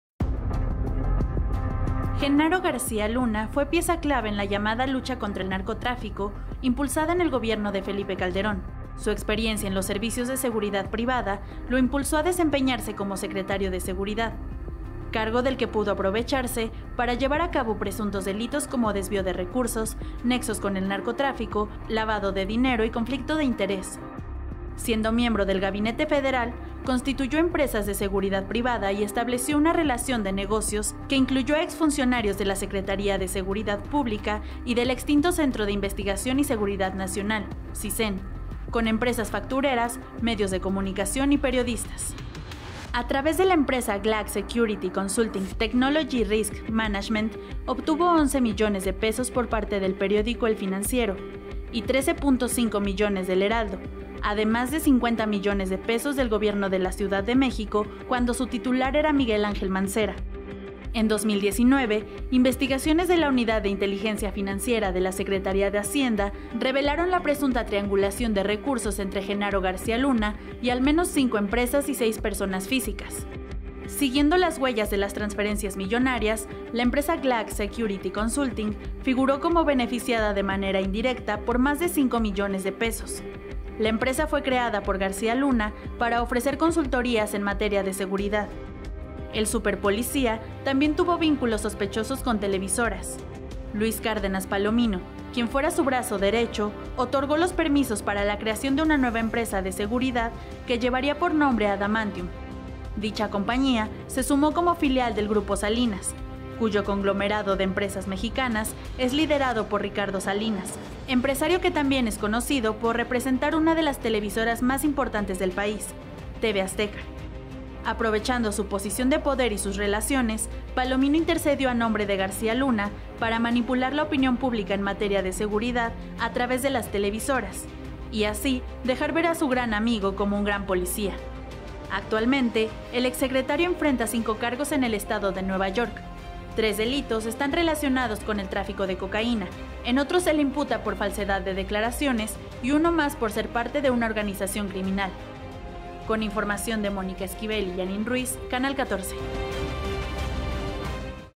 Triangulación de empresas de seguridad por García Luna
Genaro García Luna, ex secretario de seguridad en el sexenio de Calderón, presuntamente creó empresas de seguridad privada que habrían sido contratadas por dependencias gubernamentales para triangular recursos.